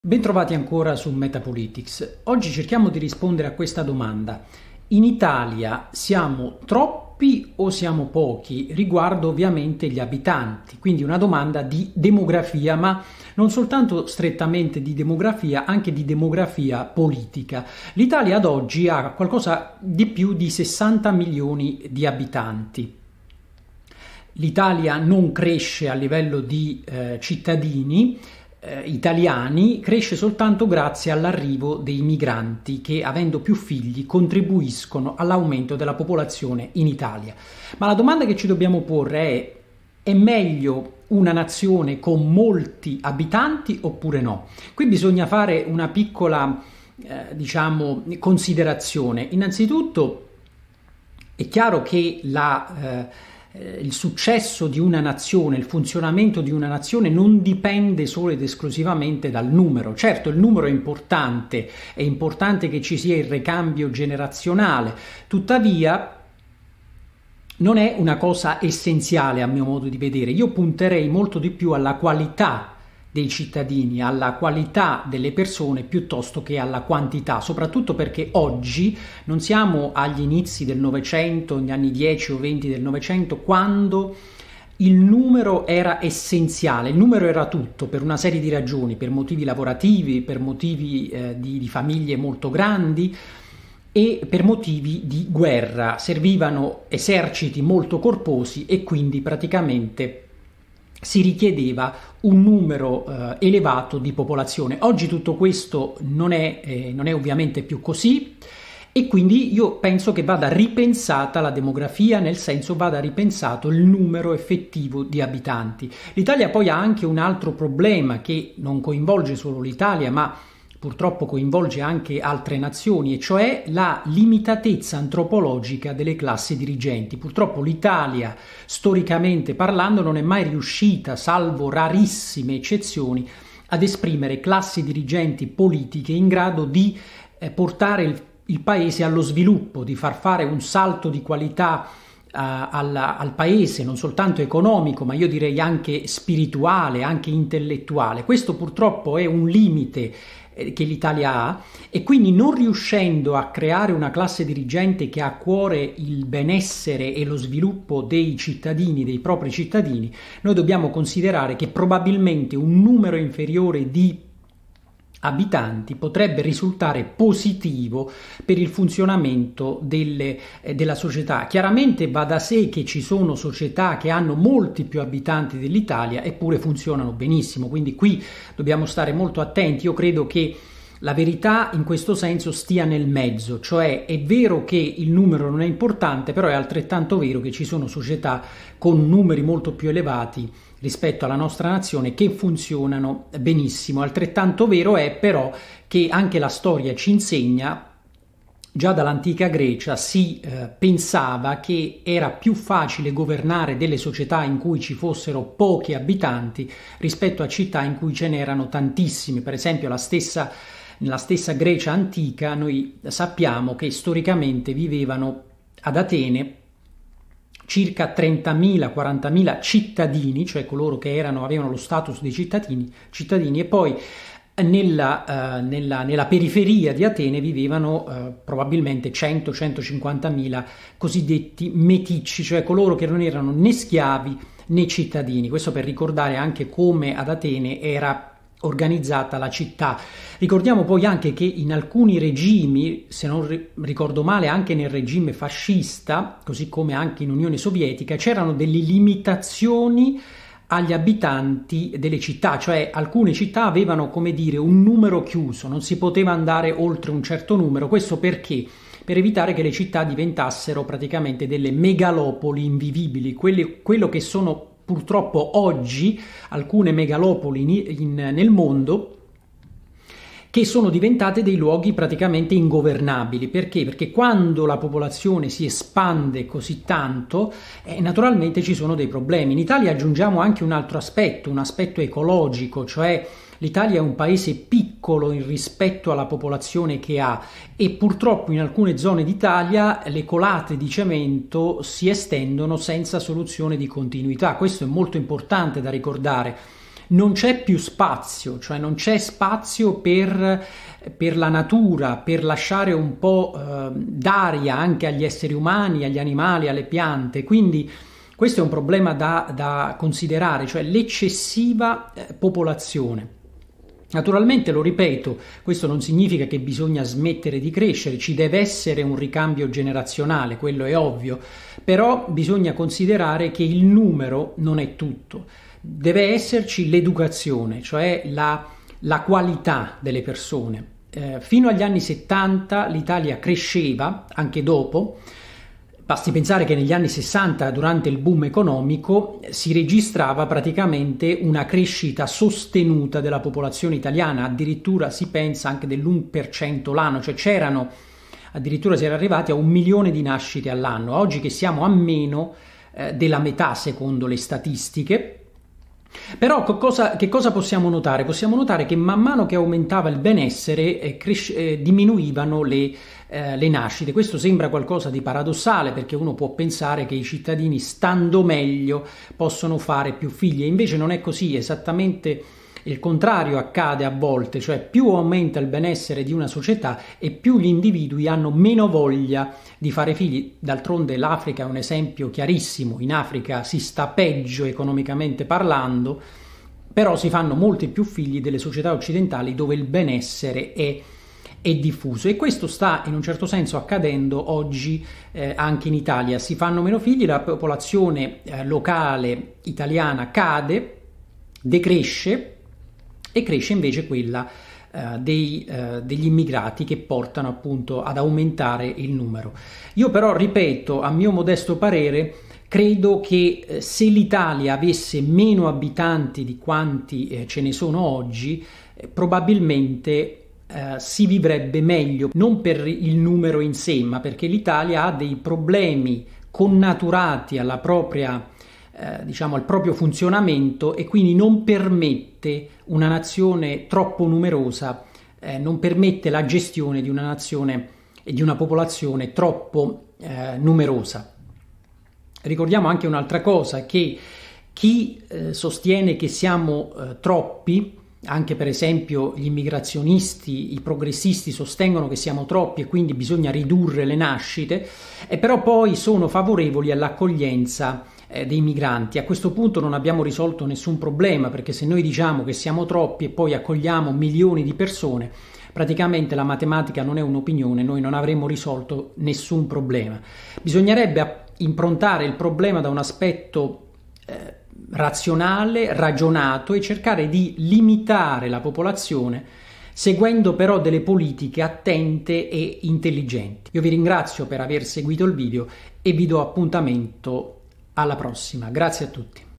Demografia e popolazione: in Italia siamo troppi o pochi?
L'Italia conta poco più di 60 milioni di abitanti. Le statistiche ci mostrano che la popolazione italiana cresce solo per via degli immigrati, mentre si registra un progressivo aumento della popolazione anziana. Al di là di tutto questo, in Italia ci sono troppi abitanti, oppure troppo pochi? Questo video prende le mosse da questo quesito, per cercare di fare una analisi del problema demografico in italia.